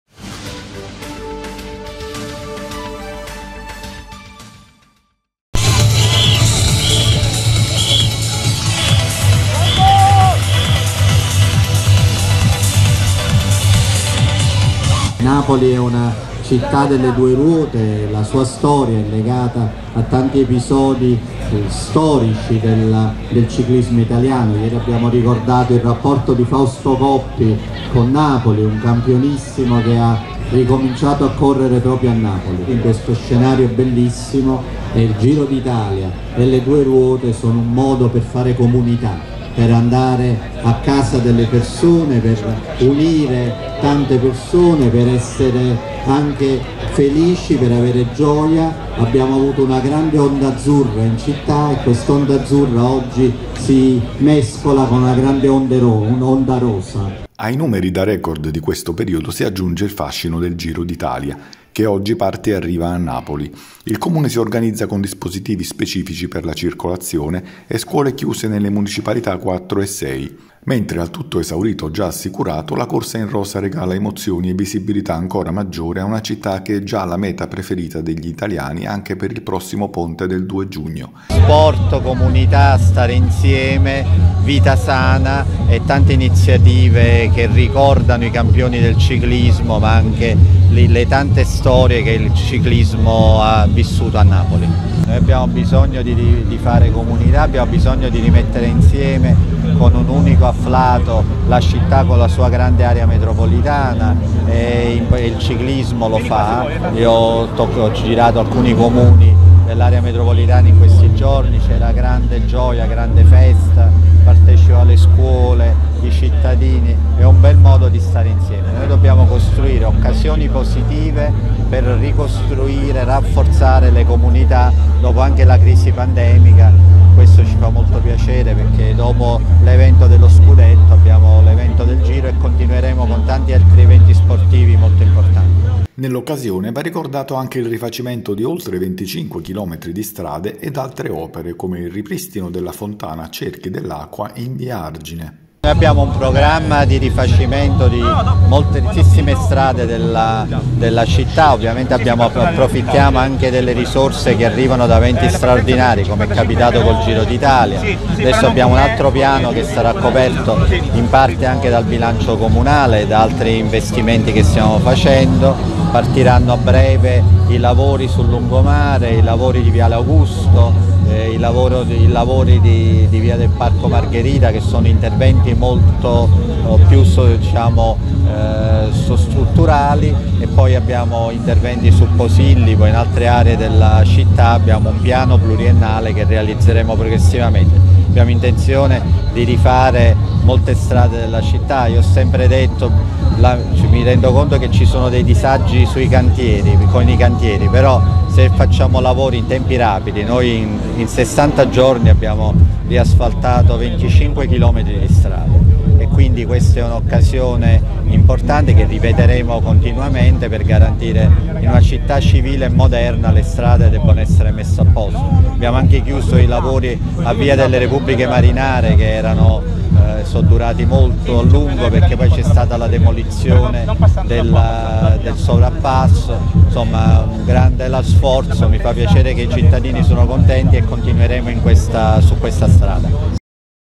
Il Giro d`Italia in una Napoli da record
Oggi il Giro d'Italia parte e arriva a Napoli, dopo aver attraversato alcuni dei panorami più belli della provincia. Emozioni e visibilità ancora maggiore a una città che è già la meta preferita dagli italiani anche per il prossimo ponte del 2 giugno.